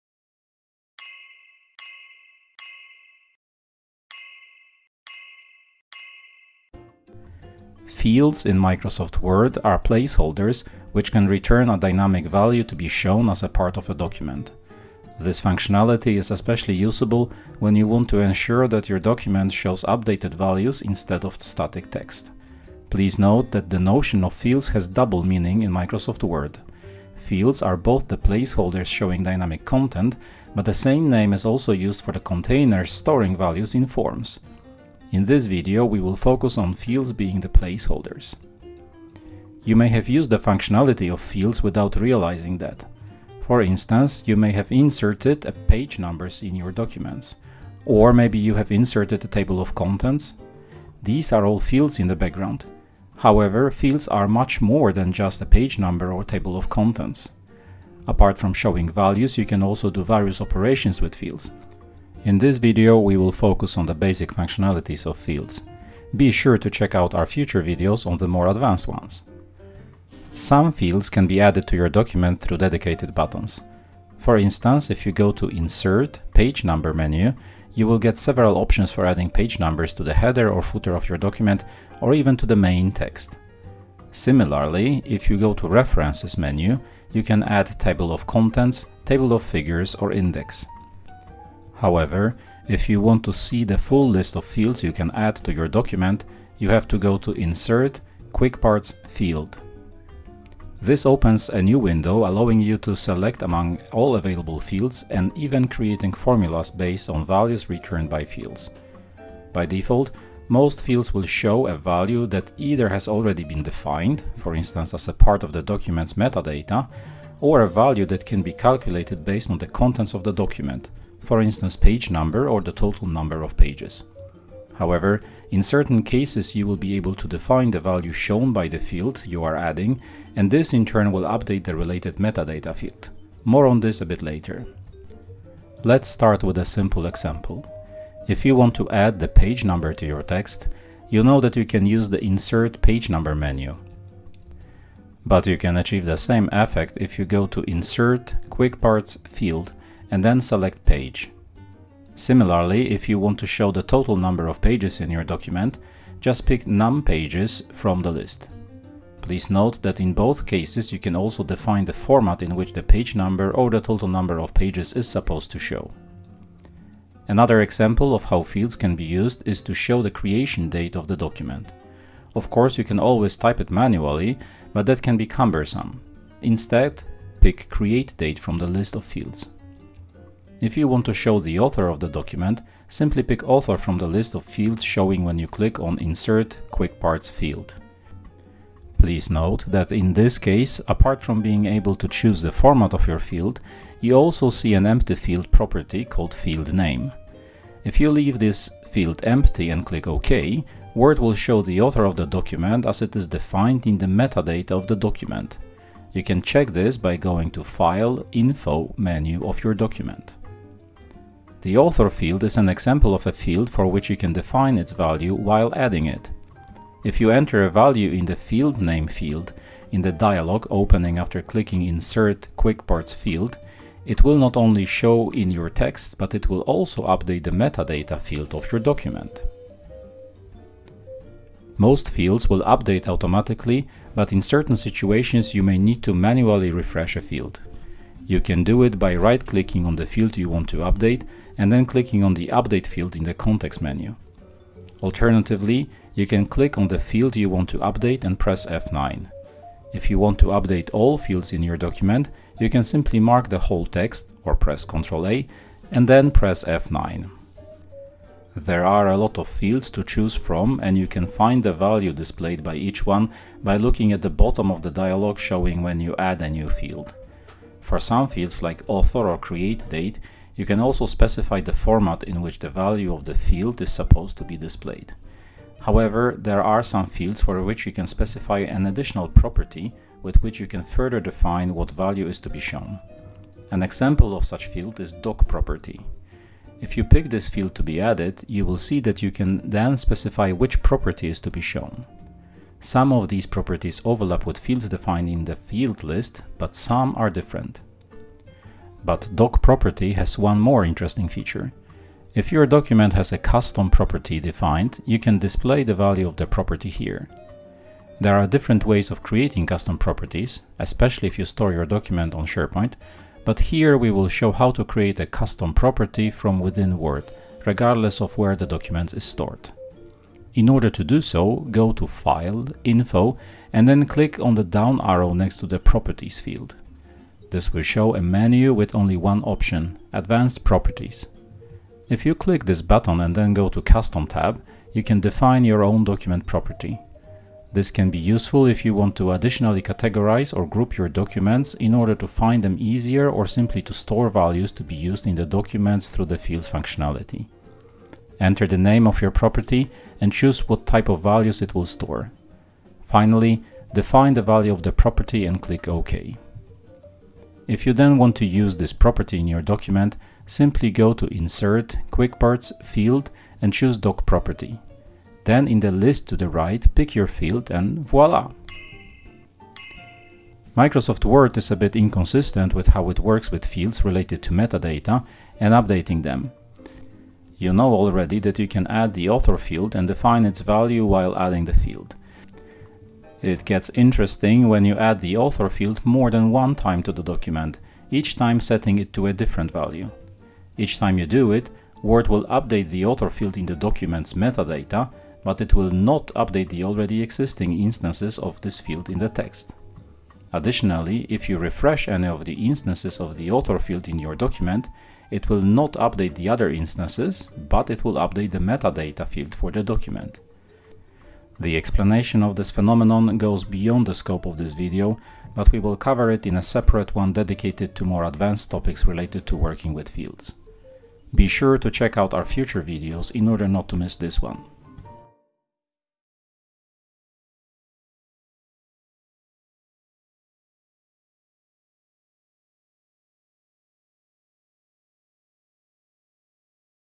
Working with fields in Microsoft Word
Microsoft Word is a great tool for creating documents - the richness of formatting options gives you all the tools you need for making your creations look fabulous. Documents are usually perceived as static pieces of text - in the end, what you write, stays unchanged.

There are though some occasions where instead of explicitly typing a value you would rather have a dynamic value inserted. The most common case is page numbering. You can of course manually number each page but most people use the Insert-Page Number functionality provided by Word. Thanks to this you do not have to manually enter numbers on each page.

Page Numbers, Tables of Contexts or Indexes are examples of Fields. Fields are placeholders showing dynamics values calculated by Word or copied from document's metadata. By using fields, you can greatly improve your productivity - metadata fields like Author or Create Date are available with just a few clicks.